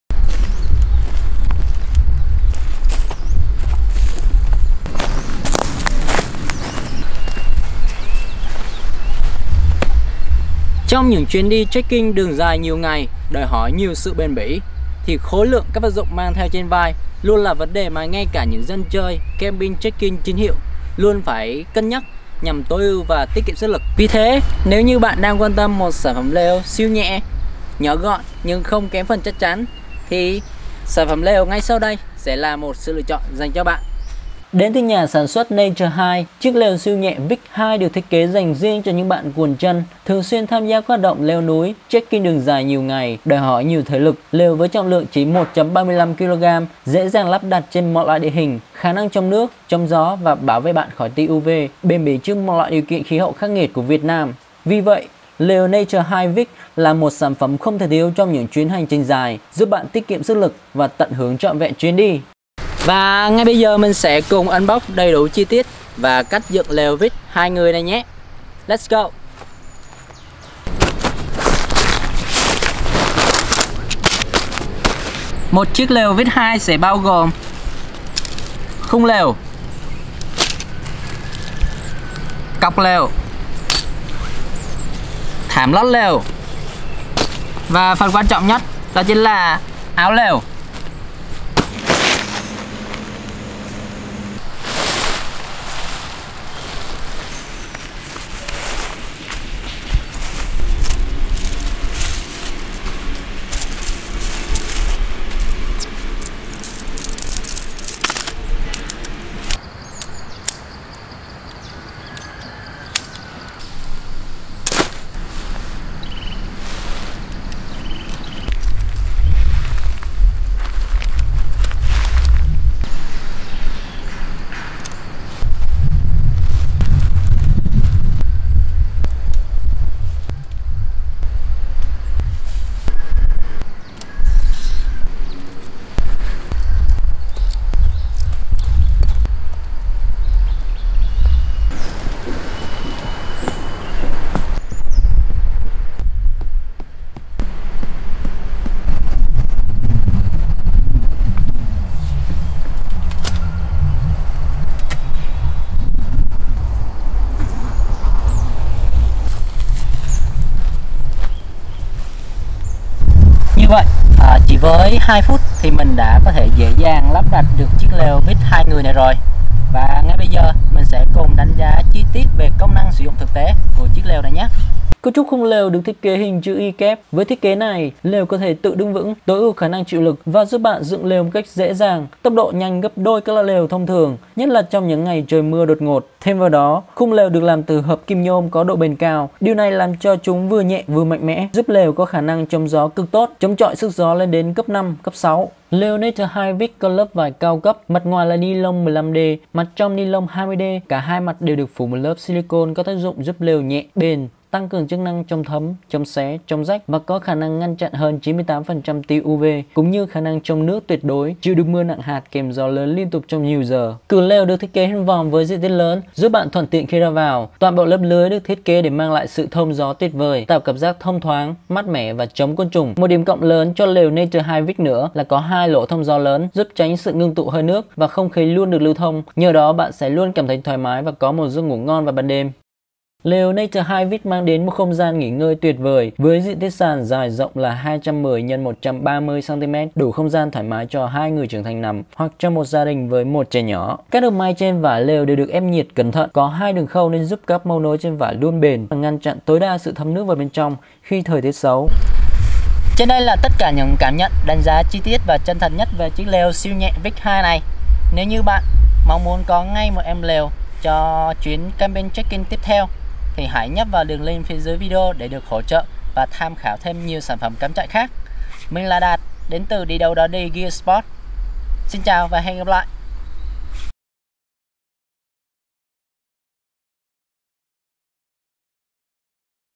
ĐÁNH GIÁ CHI TIẾT LỀU CẮM TRẠI 2 NGƯỜI | SIÊU NHẸ, CHỐNG GIÓ, CHỐNG NƯỚC | ĐI ĐÂU ĐÓ ĐI
Lều du lịch 2 người Naturehike Vik NH19ZP003-1 với trọng lượng 1.35kg cho người đi bộ đường dài nhiều ngày, dễ dàng lắp đặt trên mọi địa hình, khả năng chống nước, chống gió và bảo vệ bạn khỏi tia UV, bền bỉ trước mọi điều kiện khí hậu khắc nghiệt của Việt Nam. Hôm nay DIDAUDODI sẽ cùng bạn đập hộp và đánh giá chi tiết về chiếc lều du lịch siêu nhẹ 2 người này nhé.

Xem nhiều sản phẩm hơn tại: didaudodi.com